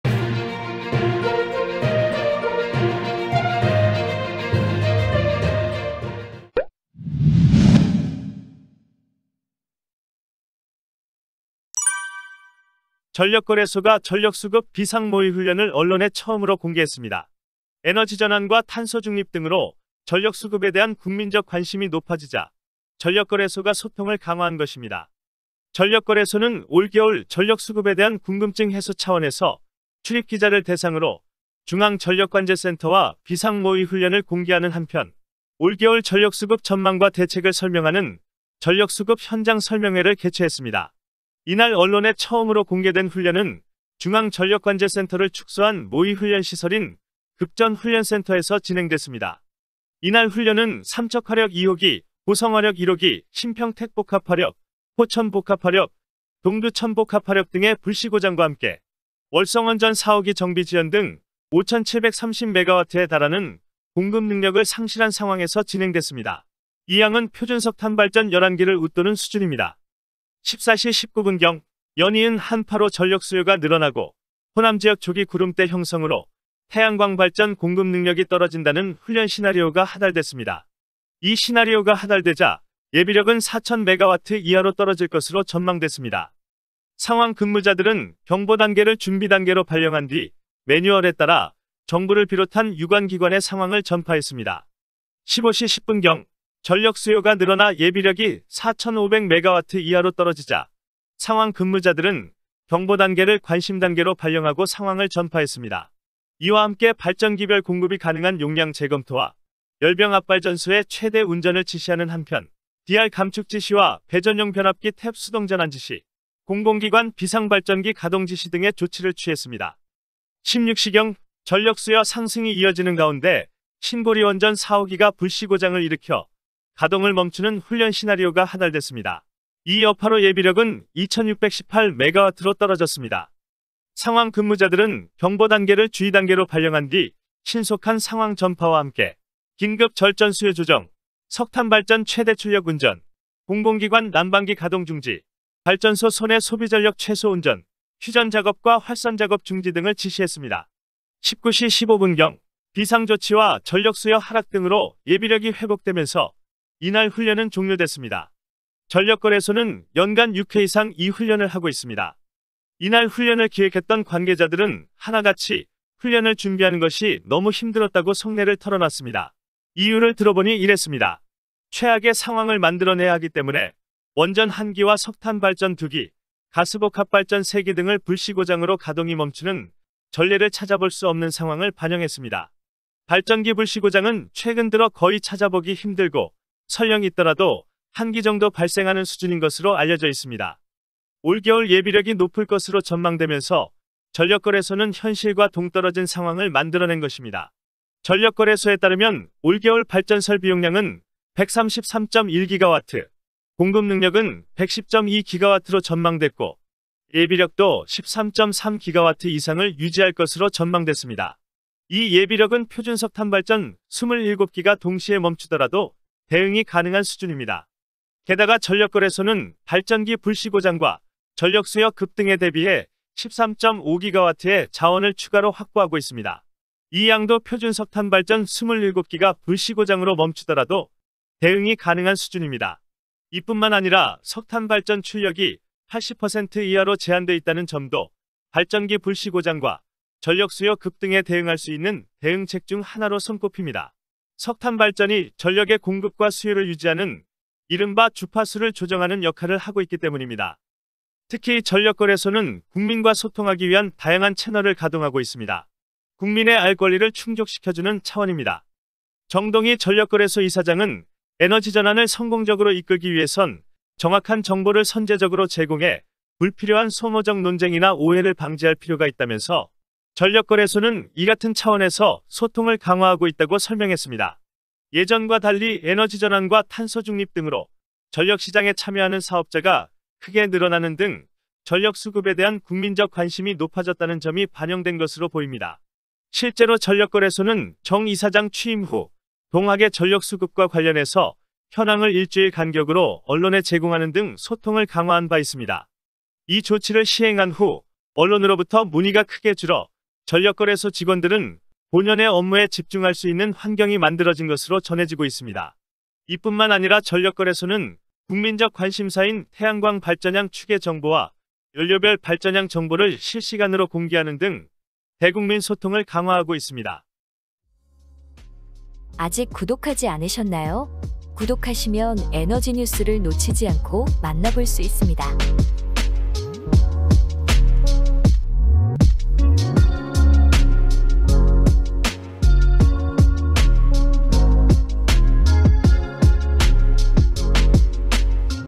올겨울 전력수급 어떻길래?…훈련보다 훈련준비가 더 힘든 이유는?
(2021년 12월 8일) 전력거래소(이사장 정동희)가 올겨울 전력수급에 대한 궁금증 해소를 위해 출입기자를 대상으로 중앙전력관제센터와 비상모의훈련을 공개하는 한편 올겨울 전력수급 전망과 대책을 설명하는 전력수급 현장 설명회를 개최했습니다.